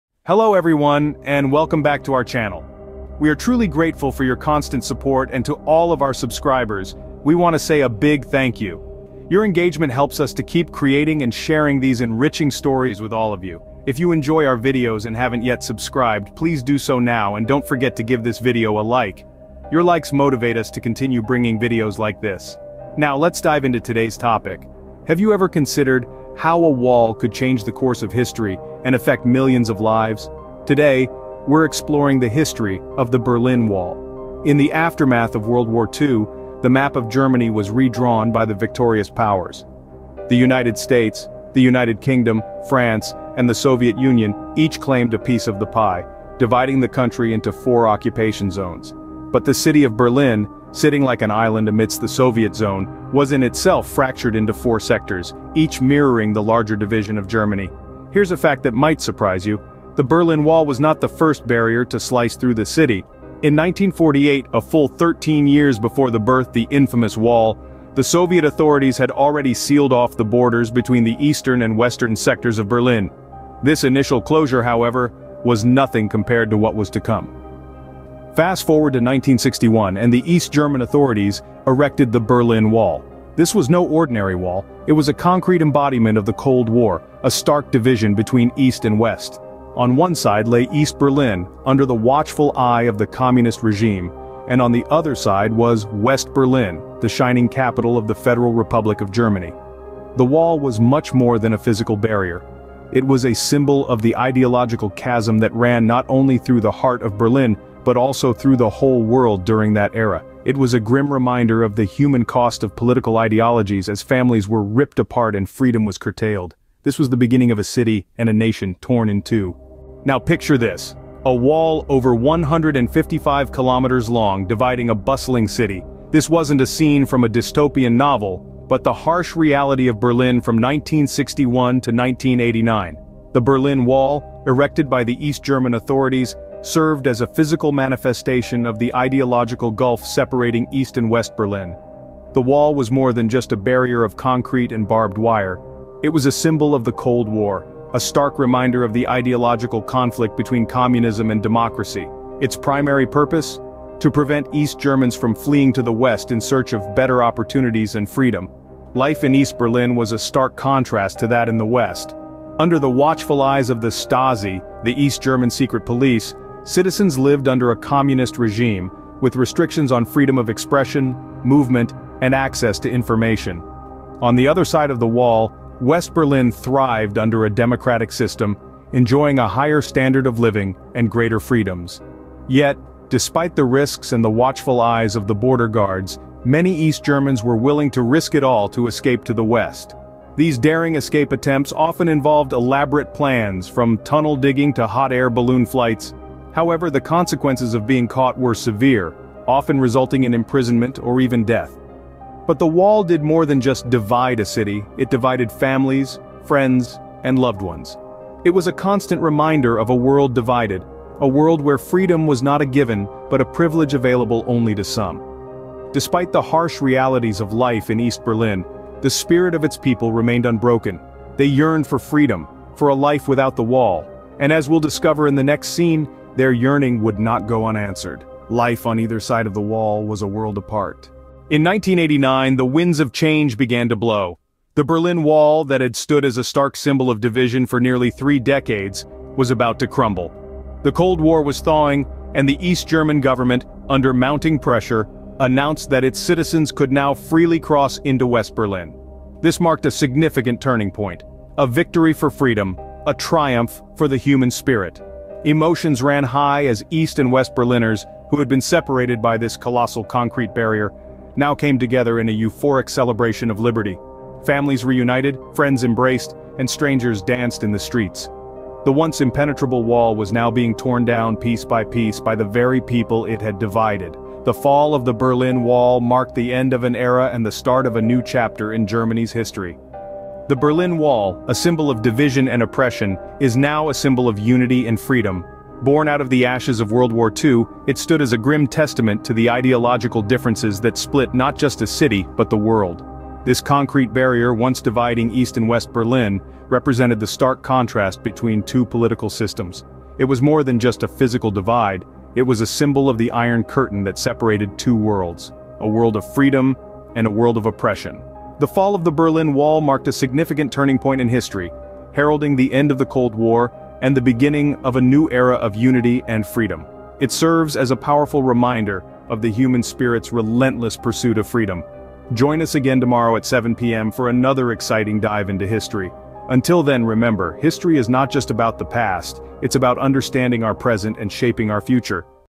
The Berlin Wall: A Symbol of Division
OUTLINE:
00:00:00 The Birth of the Berlin Wall
00:01:59 The Wall in Action
00:03:51 The Fall of the Wall
00:04:40 Conclusion